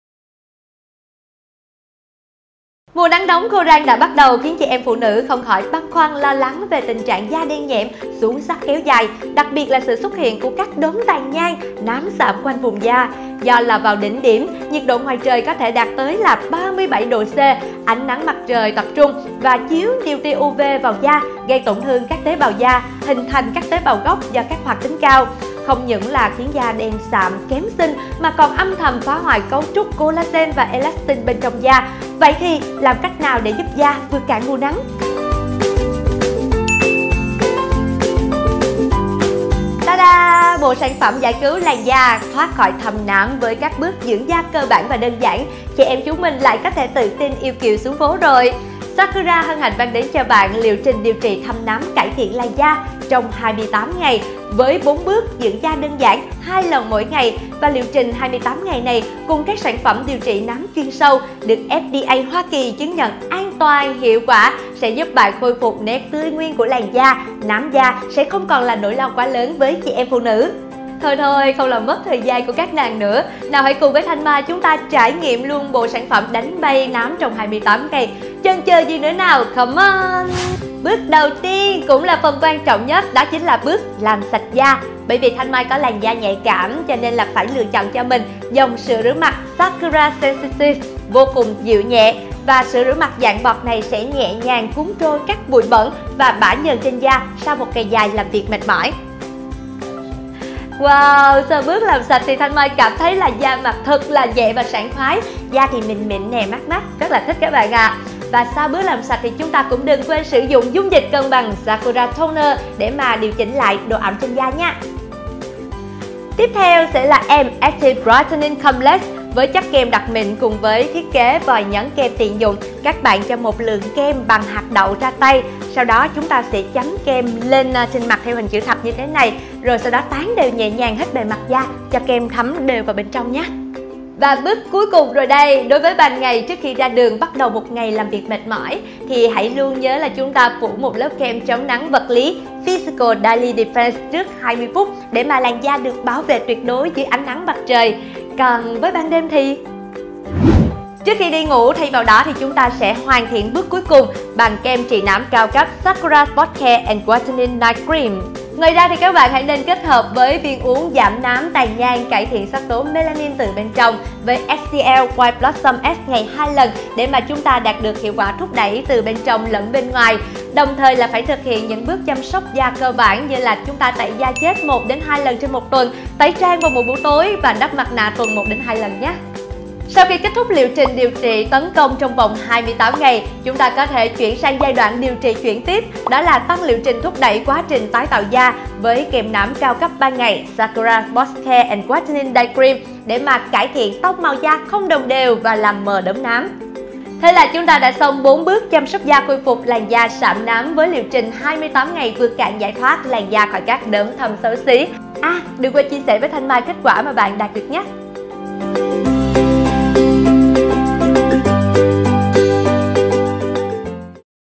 Review Liệu trình sản phẩm Sakura giúp làm mờ thâm nám chỉ sau 28 ngày
Bạn có thể mua sản phẩm chính hãng tại Hệ Thống Cửa Hàng Làm Đẹp:
Số điện thoại mua hàng: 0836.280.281 Hoặc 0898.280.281